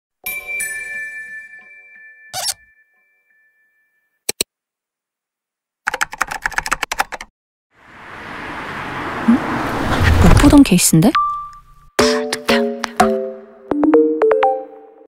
광고 캐스퍼 프리런칭 광고 '사전클릭' 편 CASPER Pre Launching, 15s
음…? 못 보던 케이스인데? 🤔

다들 저 기다리셨죠? 😎
case by CASPER, 지금 사전 클릭 중!

캐스퍼가 만들어 줄 새로운 케이스들,
가장 먼저 경험하고 싶다면 지금 얼리버드 예약하세요. 🏃‍♂🏃‍♀🏃